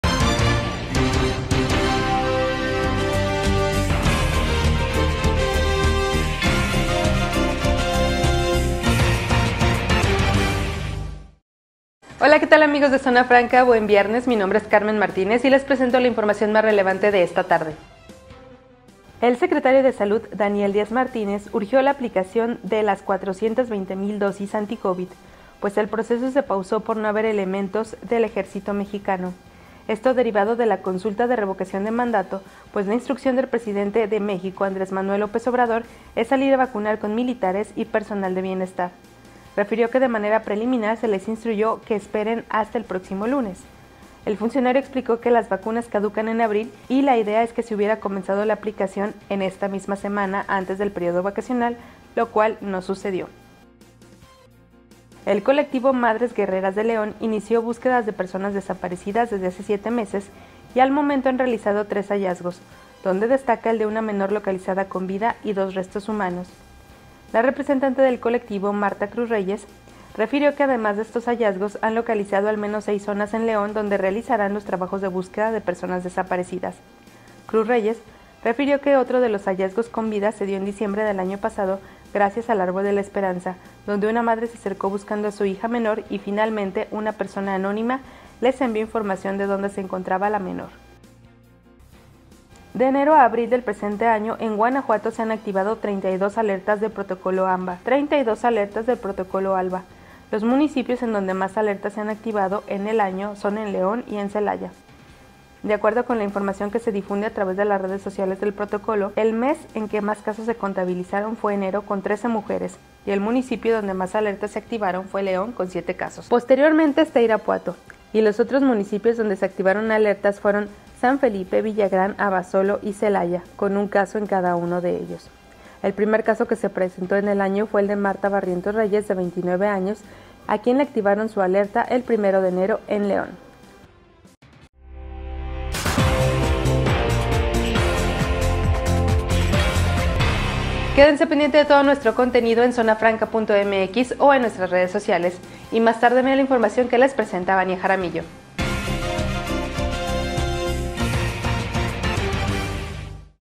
De enero a abril se han activado 32 alertas de Protocolo Alba | BrevesDeLaZona📰
Esto es LoÚltimo que ha pasado en Guanajuato
- Retrasa consulta aplicación de vacunas en Guanajuato, están desde el lunes
- Madres Guerreras de León han realizado tres hallazgos en siete meses
- De enero a abril se han activado 32 alertas de Protocolo Alba; sus familias las buscan